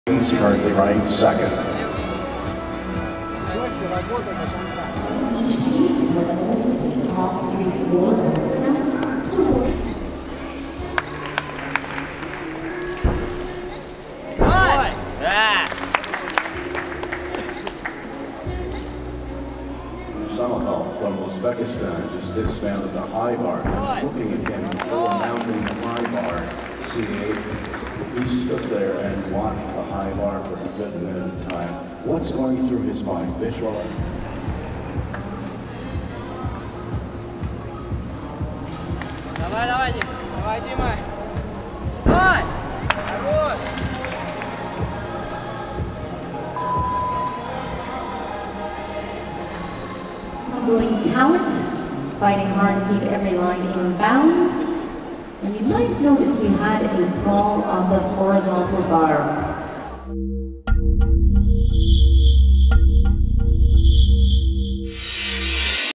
2018 Artistic Worlds – Russian men go big on Floor – We are Gymnastics!
FIG Official – 48th Artistic Gymnastic Gymnastics World Championships, Doha (Qatar), 25 October - 3 November 2018.  Russia's has not one but two gymnasts, Nikita Nagornyy and Dmitrii Lankin, risking a triple back on Floor Exercise. They both landed the H-rated skill during qualifications.